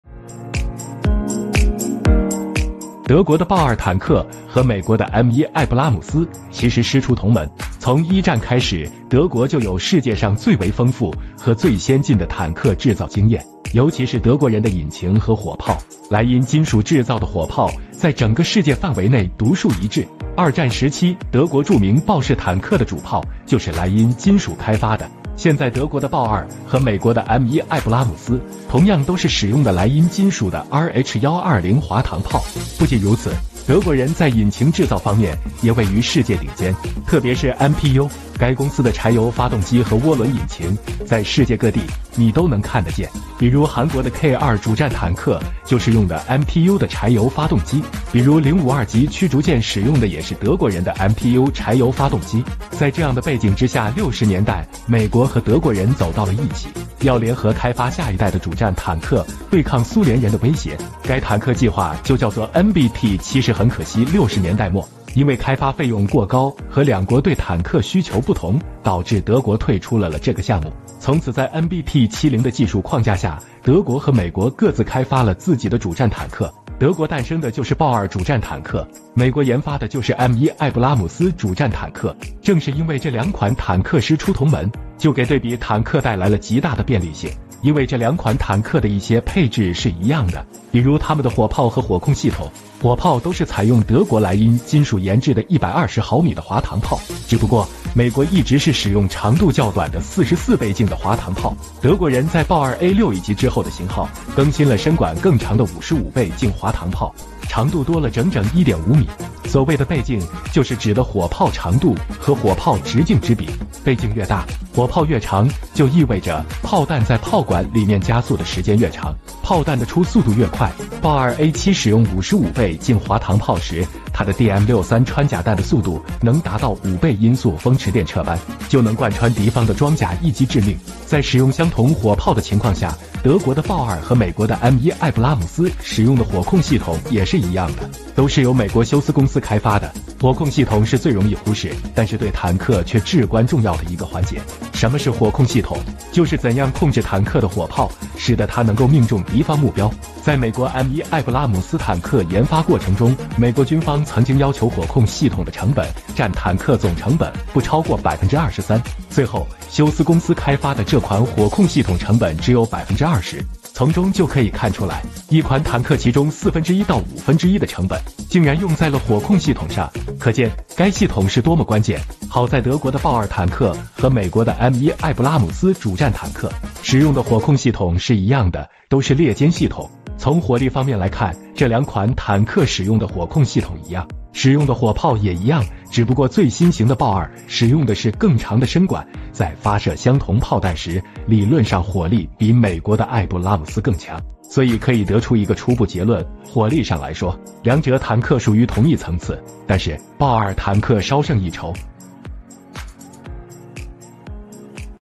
美国M1与德国豹2火力比较 为什么说豹2坦克要稍胜一筹？
介绍武器装备，科普军事知识